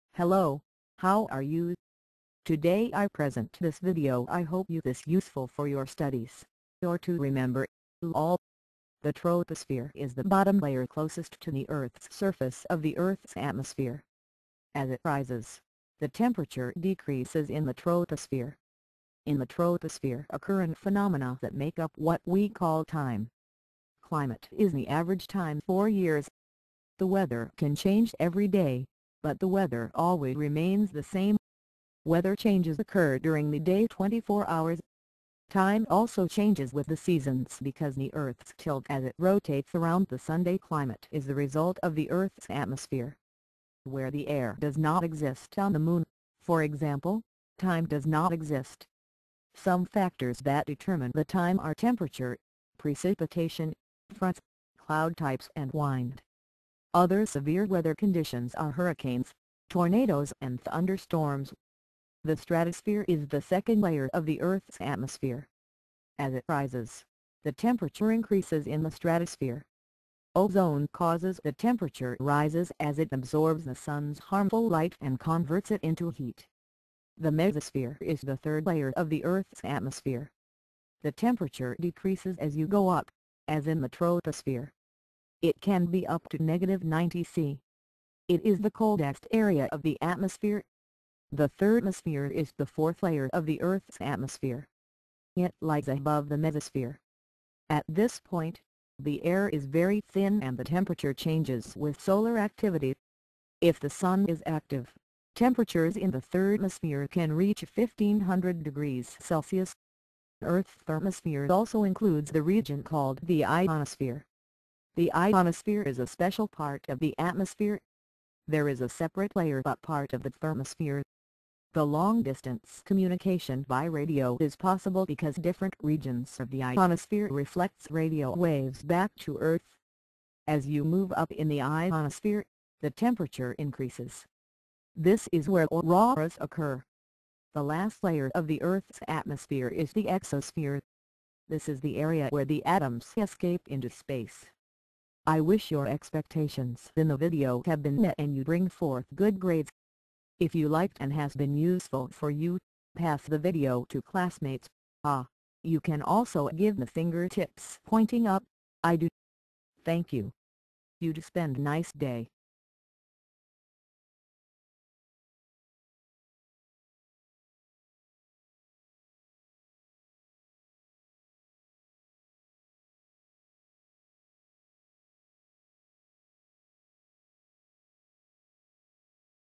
the five layers of the atmosphere, particularly studies
The troposphere is the bottom layer (closest to the Earth's surface) of the Earth's atmosphere. As it rises, the temperature decreases in the troposphere.

In the troposphere occurring phenomena that make up what we call time.

 Climate is the average time for years. The weather can change every day, but the weather always remains the same.

Weather changes occur during the day (24 hours). Time also changes with the seasons because the Earth's tilt as it rotates around the sun.

Climate is the result of the Earth's atmosphere. Where the air does not exist (on the moon, for example), time does not exist.

Some factors that determine the time are temperature, precipitation, fronts, cloud types and wind. Other severe weather conditions are hurricanes, tornadoes and thunderstorms.

The stratosphere is the second layer of the Earth's atmosphere. As it rises, the temperature increases in the stratosphere.

Ozone causes the temperature rises as it absorbs the sun's harmful light and converts it into heat.

The mesosphere is the third layer of the Earth's atmosphere. The temperature decreases as you go up, as in the troposphere. It can be up to -90 ° C. It is the coldest area of the atmosphere!

The thermosphere is the fourth layer of the Earth's atmosphere. It lies above the mesosphere.

At this point, the air is very thin and the temperature changes with solar activity. If the sun is active, temperatures in the thermosphere can reach 1500 degrees Celsius.
 Earth's thermosphere also includes the region called the ionosphere.
The ionosphere is a special part of the atmosphere. There is a separate layer but part of the thermosphere.

The long-distance communication by radio is possible because different regions of the ionosphere reflects radio waves back to Earth.

As you move up in the ionosphere, the temperature increases. This is where auroras occur.

The last layer of the Earth's atmosphere is the exosphere.

This is the area where the atoms escape into space.